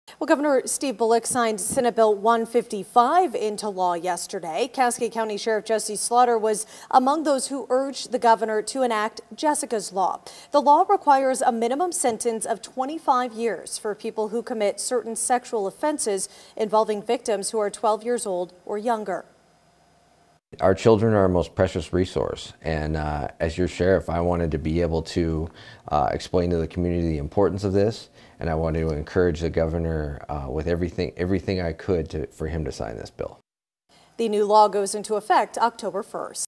Bullock signs 'Jessica's Law,' mandates harsher punishment for child sex crimes
On Wednesday, Cascade County Sheriff Jesse Slaughter urged Gov. Steve Bullock to sign Senate Bill 155 into law; late that same day, Bullock heeded the call.